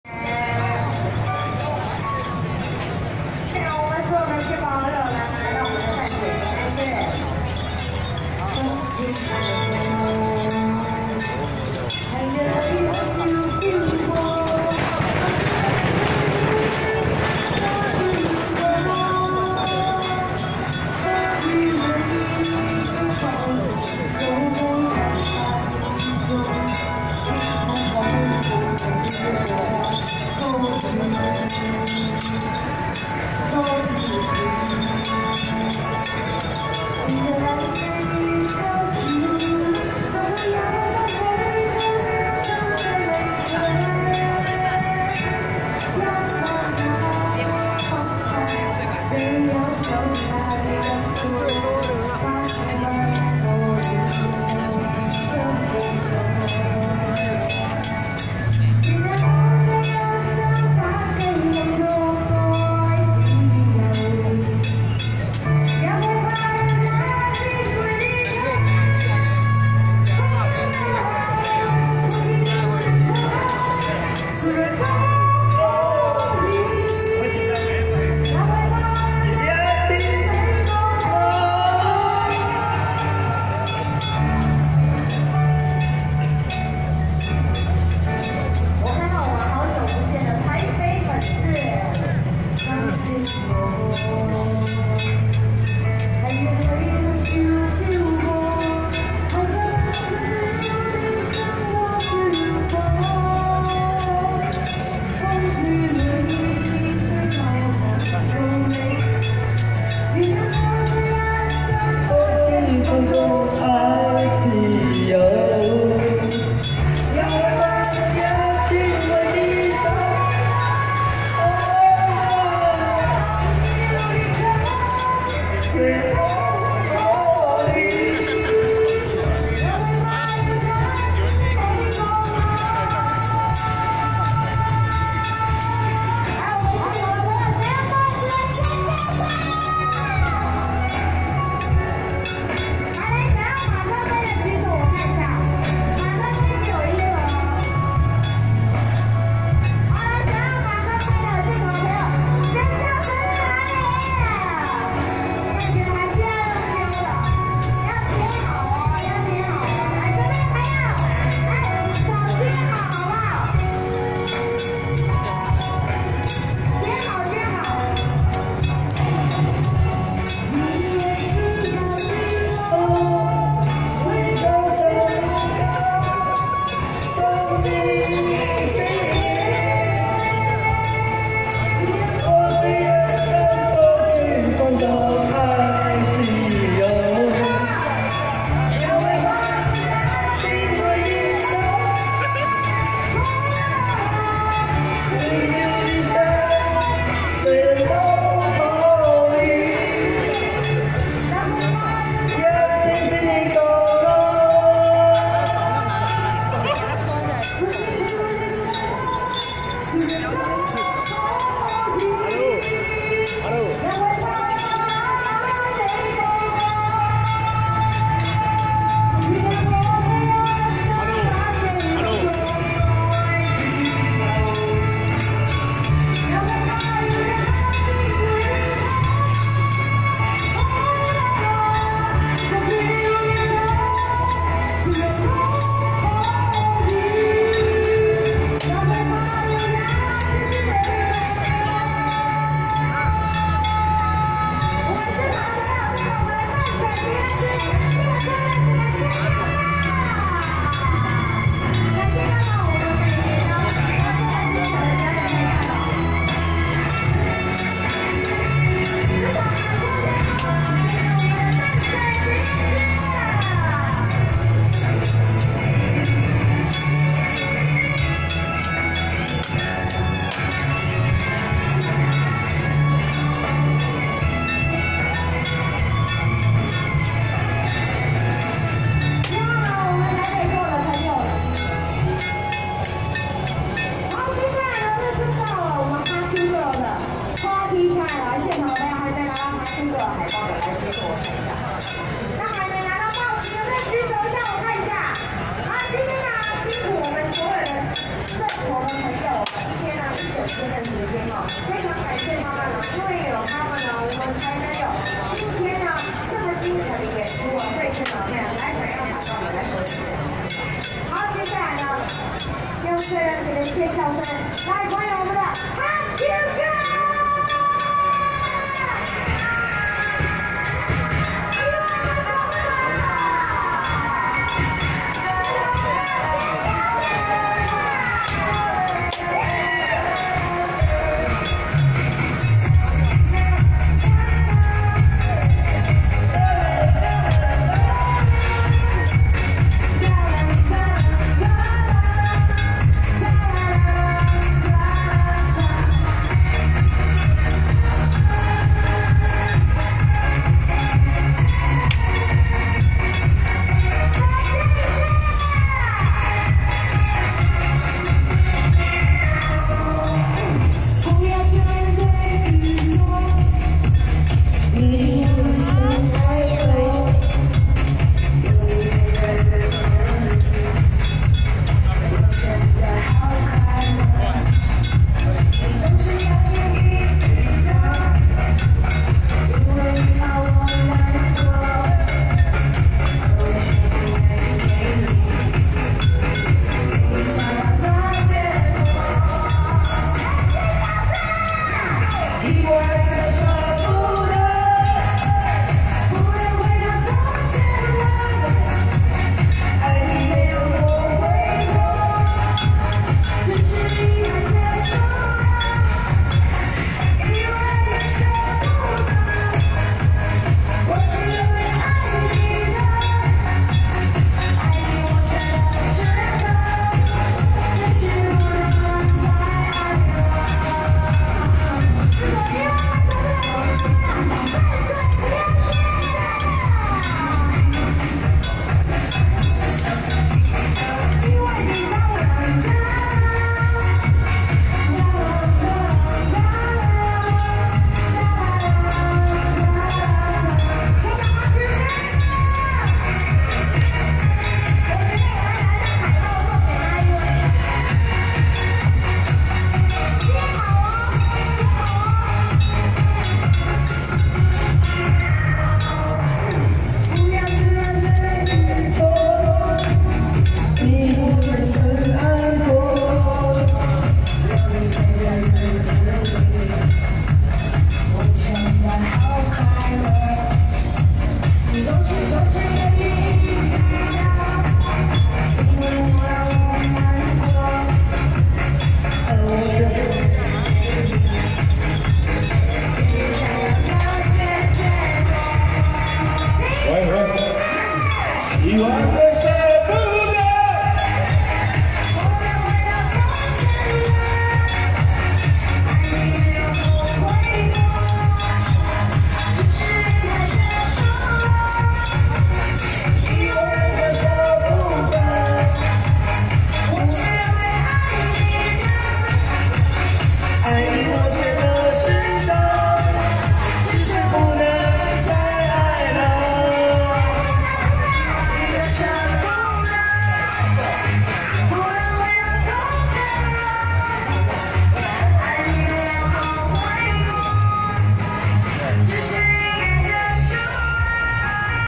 106淡水清水巖恭迎清水祖師暗訪遶境【桃園觀聯社五靈堂-八家將】
桃園觀聯社五靈堂-八家將